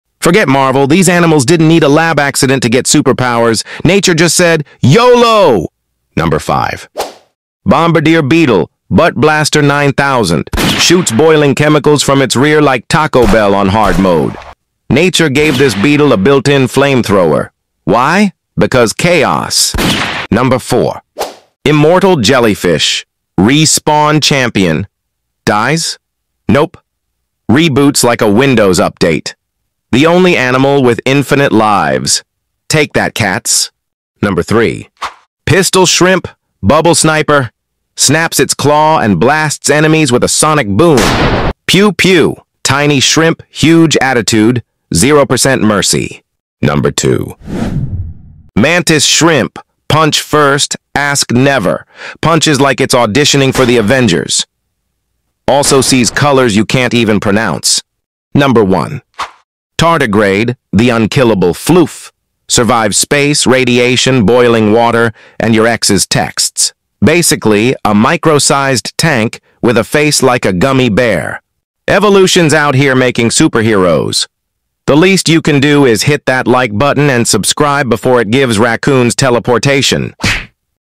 “5 Animals That Shouldn’t Be Legal 😂”
5 Animals That Shouldn’t Be Legal 😂

🦸‍♂️ Think superheroes are just in movies? Think again.
These 5 real animals have powers so insane, they make Marvel look like a documentary.
🔥 Punches like a bullet? ✔
🔥 Shoots boiling acid from its butt? ✔
🔥 Can literally live forever? ✔✔

From the pistol shrimp’s bubble blast to the immortal jellyfish’s infinite lives, nature’s out here writing its own comic book — no CGI needed.

👇 Comment your favorite animal superpower — or the one you wish you had!